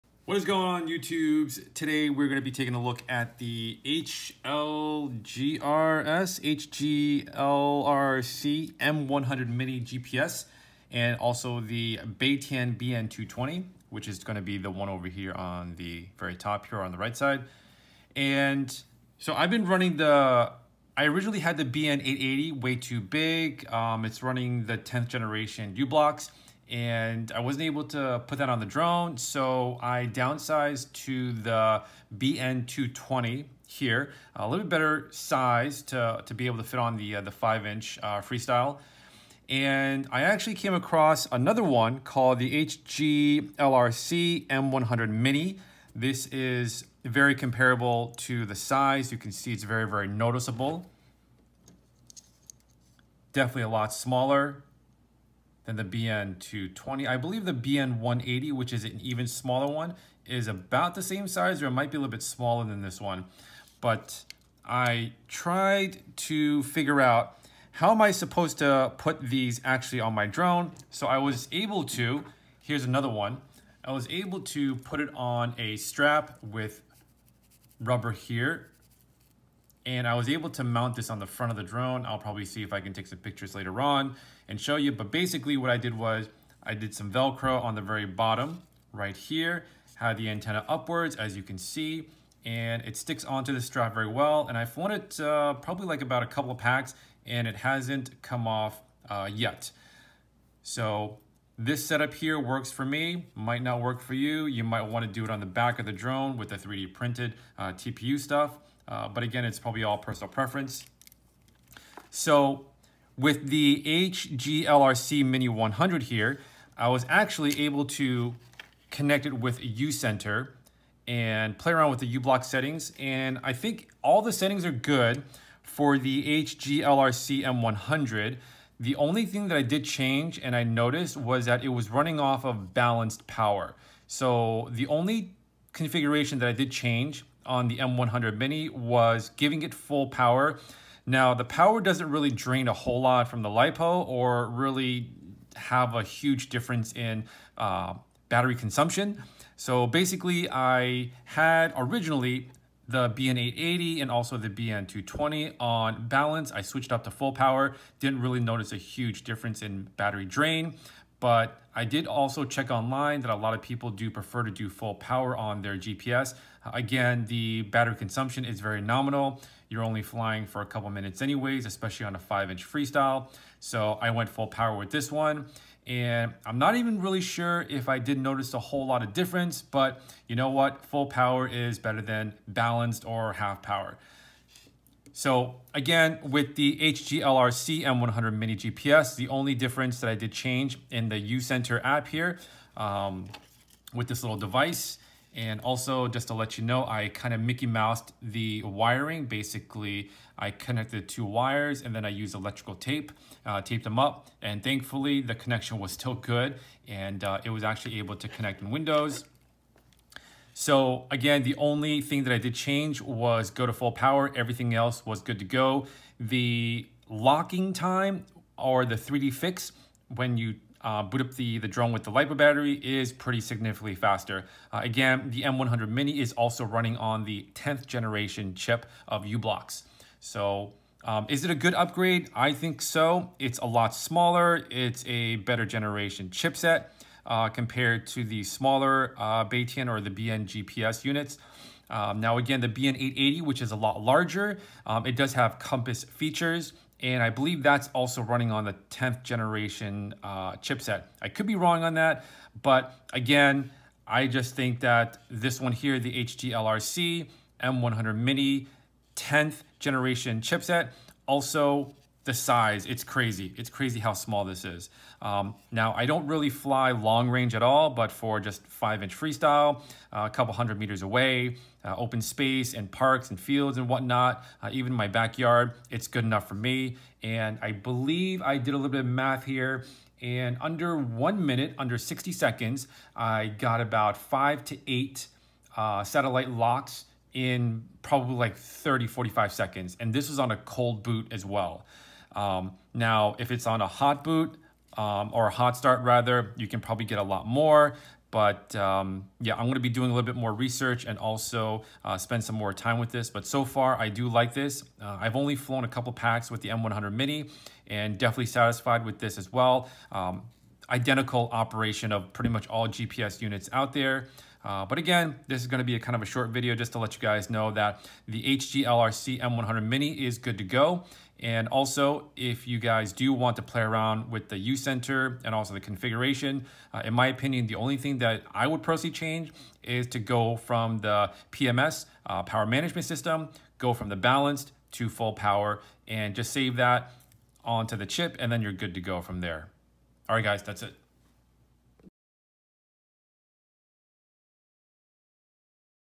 UBLOX M10 GPS - 5m review - new hotness - 5'' Freestyle GPS upgrade, it's been real M8 Module
I was on Beitian M8 GPS, and switched all my drones to M10 and not looking back, price is reasonable and the GPS is better, I like em!